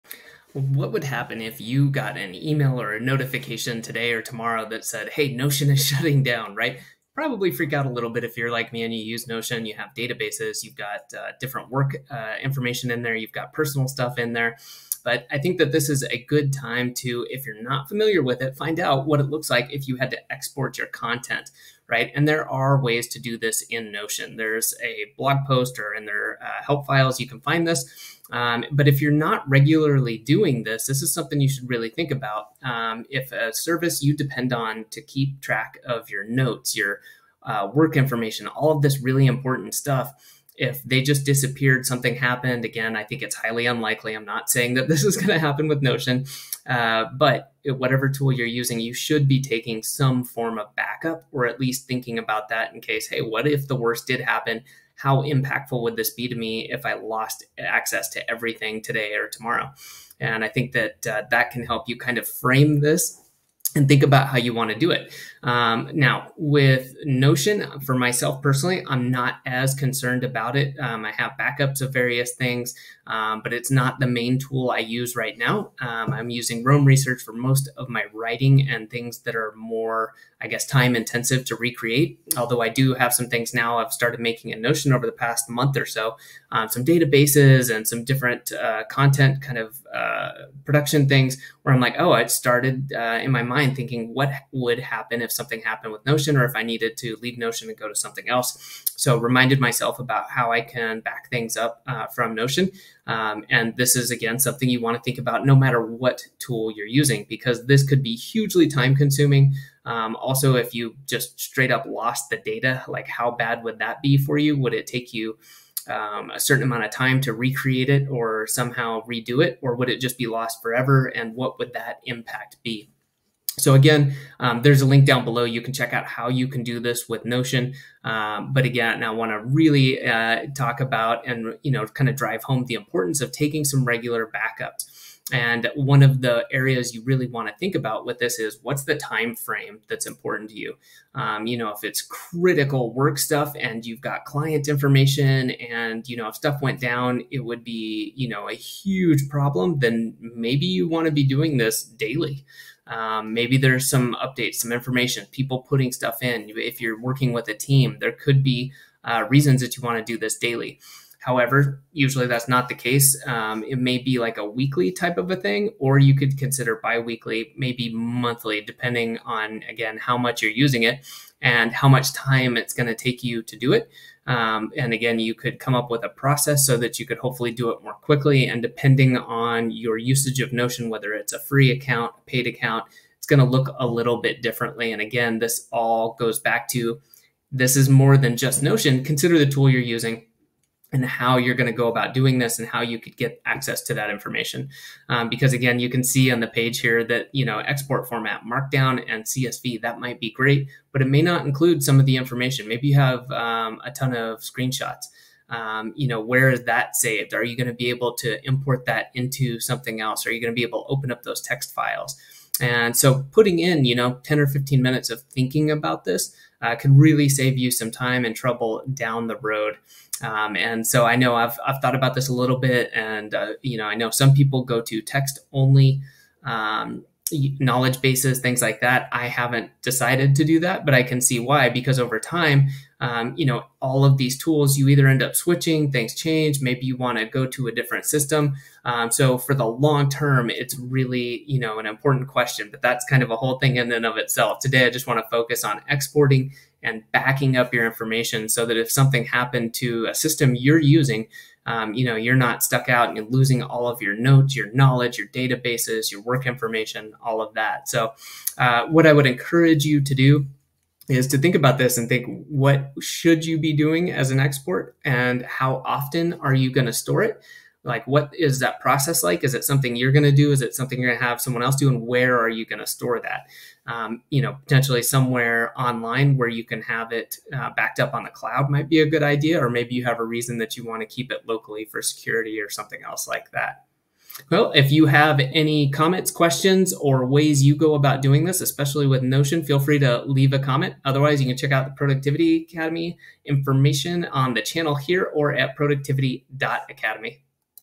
Avoid Data Disaster: What Would Happen If Notion Disappeared
Ever wondered what would happen if your go-to tool for databases, work info, and personal stuff, like Notion, suddenly shut down? 😱 In today's video, I dive into essential Notion backup tips to avoid data disaster and ensure your vital information remains safe and sound. Would your data be safe? Do you have a backup? Notion is a great tool, but make sure that you have backups no matter what tool you use!

Discover how regular backups not only safeguard your work but also contribute to effective time management and prevent huge issues down the road.

Got your own tips or questions about Notion backups? Dive into the comments below—I'd love to hear from you! And don't forget to check out the Productivity Academy for more resources on maximizing your efficiency and achieving your goals.

CHAPTERS:
0:00 - Notion Shutdown Consequences
1:02 - Notion Backup Strategy
2:12 - Notion Backup Frequency
3:15 - Notion Export Features